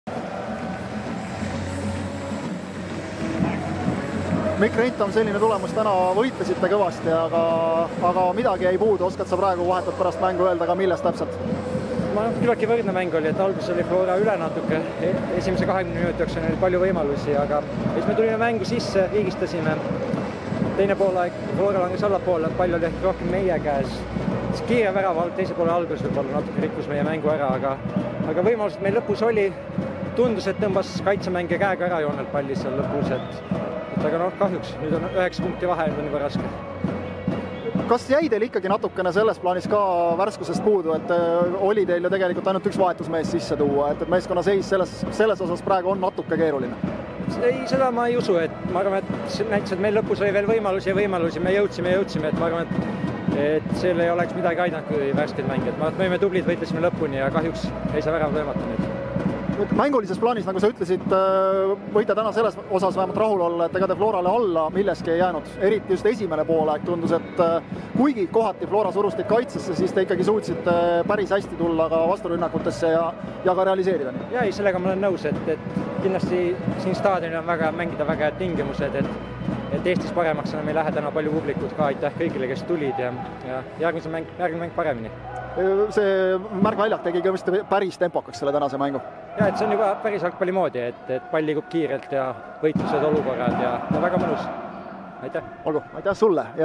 14. voor 2020: Tallinna FC Flora - Nõmme Kalju FC 2:1 (1:1), Reintami intervjuu
24. juulil kell 19.30 A. Le Coq Arenal

Tallinna FC Flora - Nõmme Kalju FC  2:1 (1:1)
19. ov. Deniss Tjapkin, 50. Rauno Sappinen - 31. Kaarel Usta